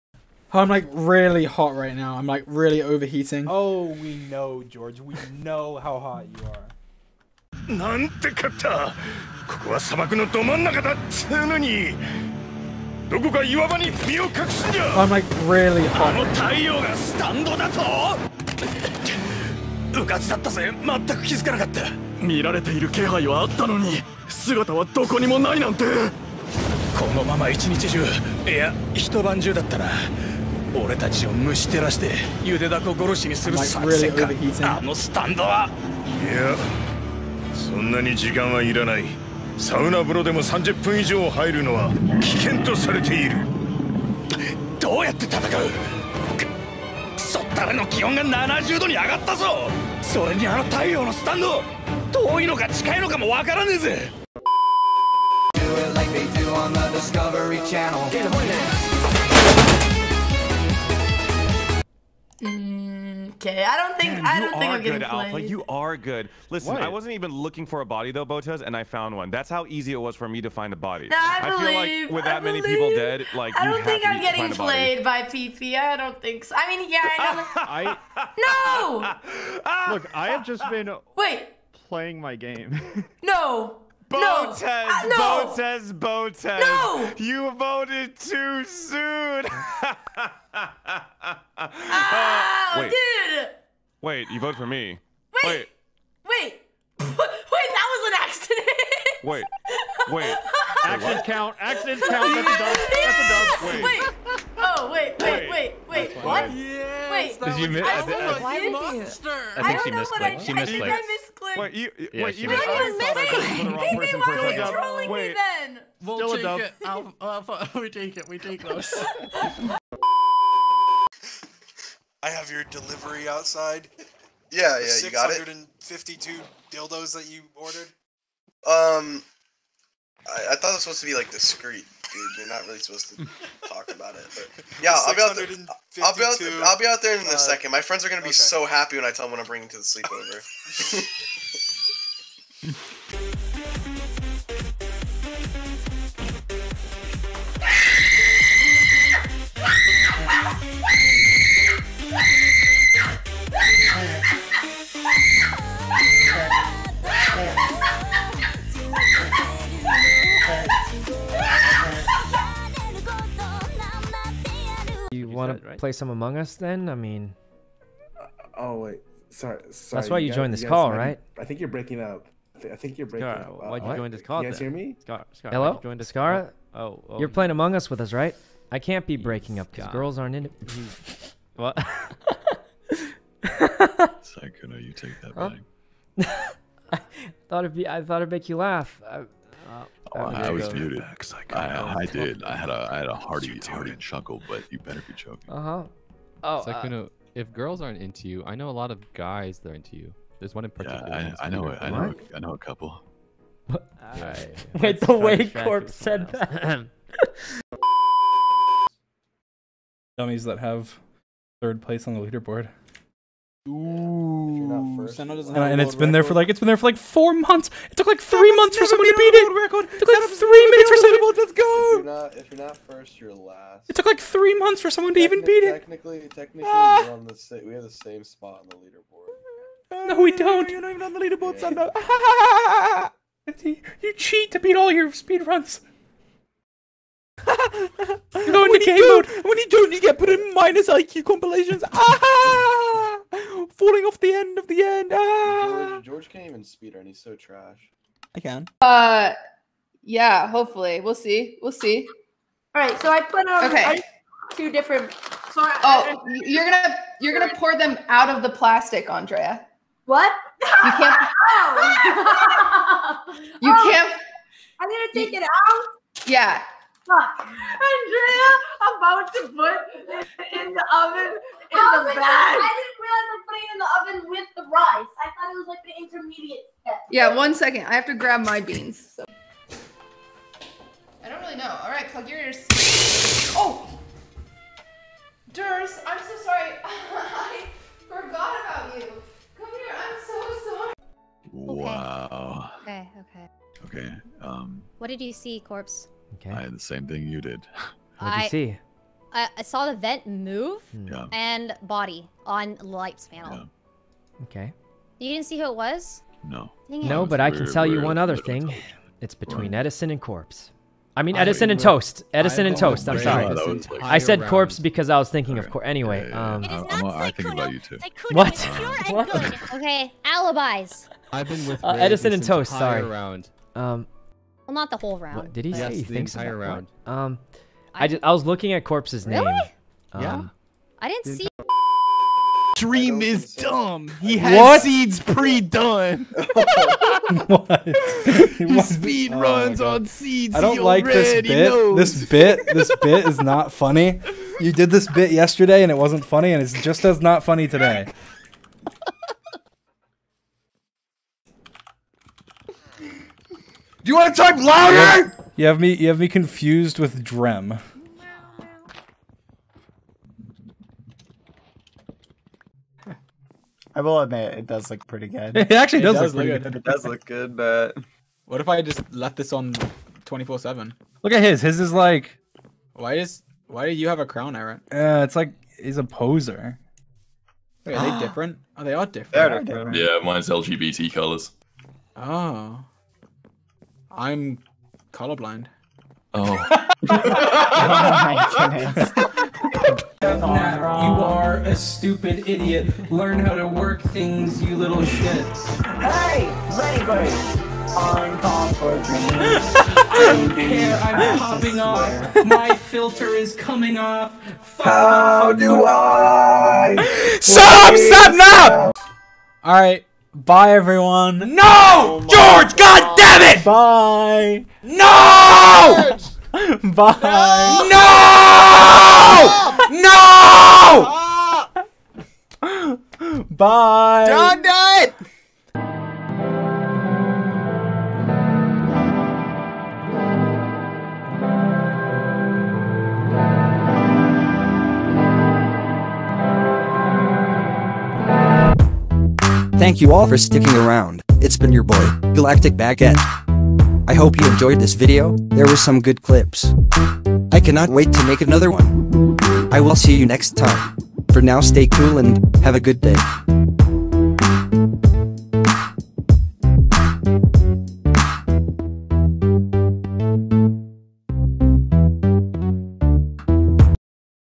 George is too hot: SUN STAND, Dream singing, Corpse and sykkuno flirting, Minx broken toe, Cooking
Well well well what do we have here. Georges stand is the sun (and next your gonna say no way they can deal with Georges hotness, he's too hot). haha hahha aahaa the new abilities of The dream dust crusaders are op aff my god will Dreams power level.

The Botezes are cooking along with QT cinderella but the struggle is real the mortal enemy of miss Andrea Botez...     PLASTIC!
the plastic is too strong.

meanwhile at the other side of the galaxy another meme-able couple. Corpse and syykuno are at it again. the hidden bromance is real. the deep love between them is showing.

George:  @GeorgeNotFound
Minx: @JustaMinx

Andrea: @botezLive

Syykuno: @syykuno
Corpse: @corpsehusband

Dream: @dreamwastaken
BadBoyHalo:  @BadBoyHalo
SNapnap:  @Sapnap